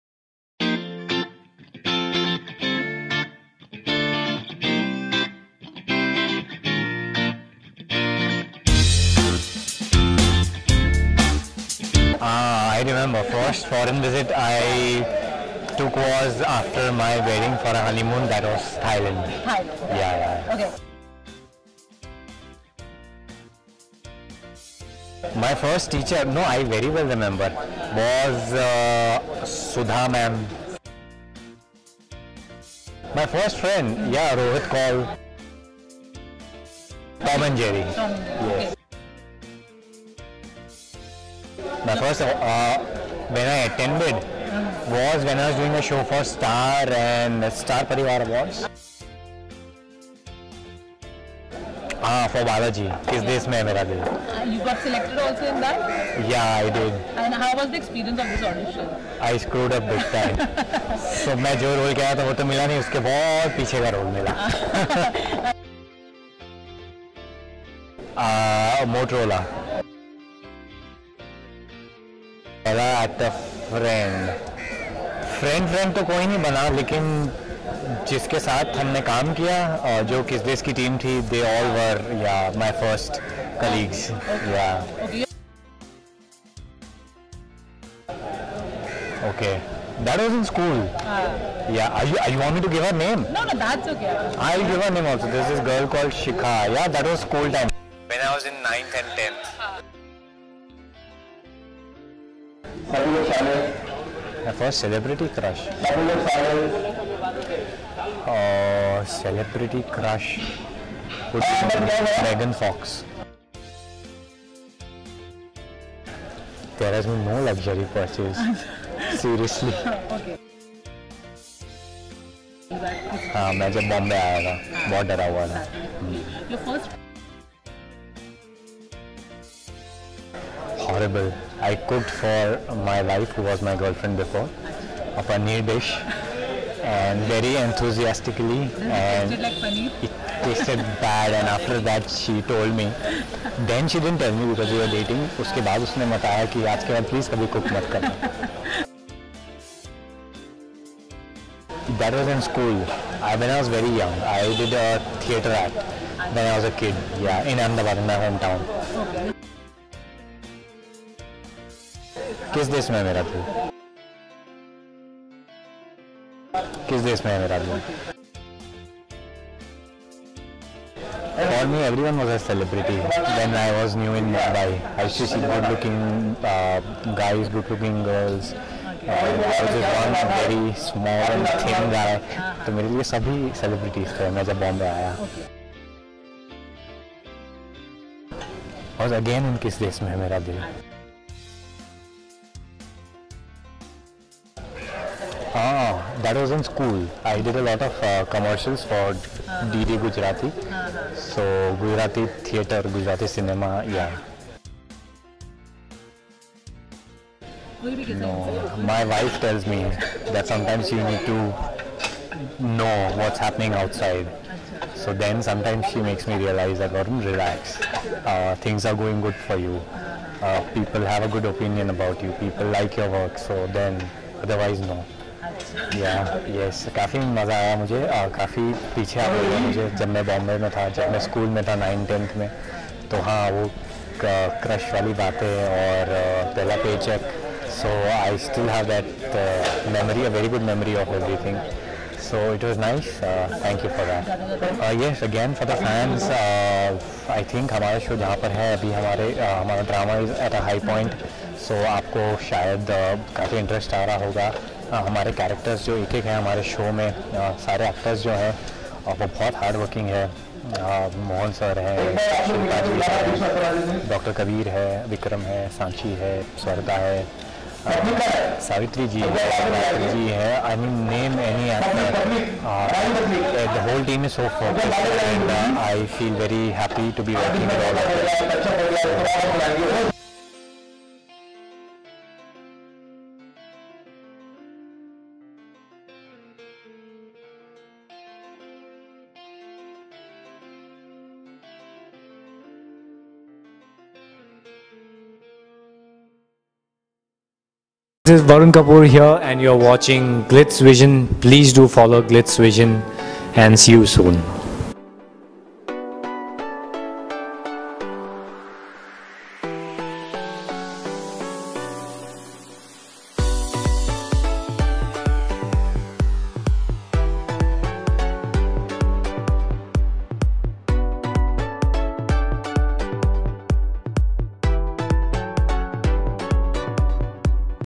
FIRST TIMES TAG with Varun Kapoor | Savitri Devi | Fun Segment
First times are always special , check out what Varun has to say about his special first(s) .